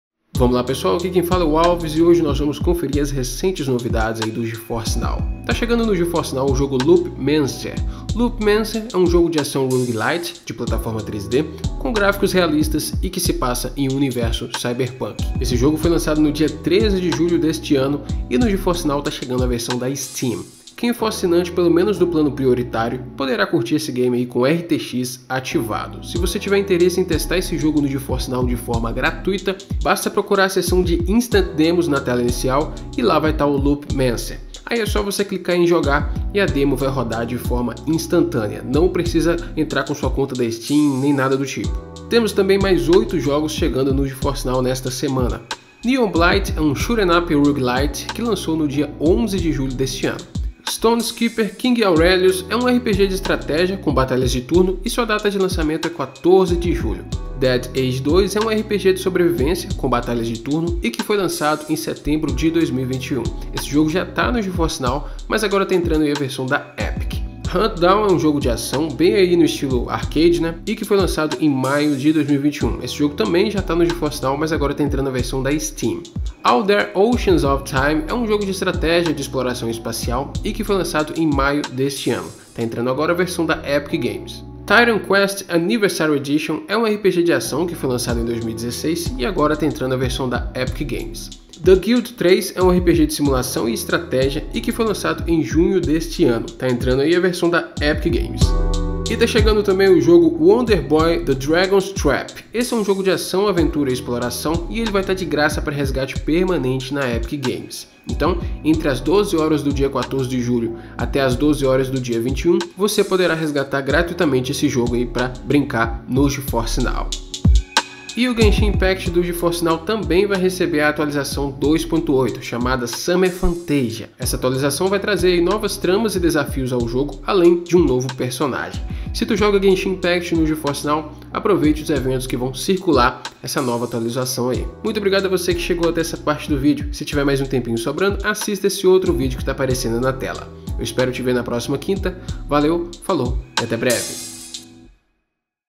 GeForce Now: Novidades no Catálogo com Jogo Grátis & Atualização Genshin Impact 2.8
Neste vídeo você verá os novos jogos que estão chegando no GeForce Now, incluindo um jogo grátis da Epic Games. Falaremos também sobre a nova atualização do Genshin Impact, a versão 2.8, chamada Summer Fantasia.

🚨Tag de Criador Epic Games:
ALLVESGAMES

0:00 Lançamento + Teste Grátis
0:42 Mais Jogos Chegando no GeForce Now
1:39 Jogos Grátis
1:58 Atualização Genshin Impact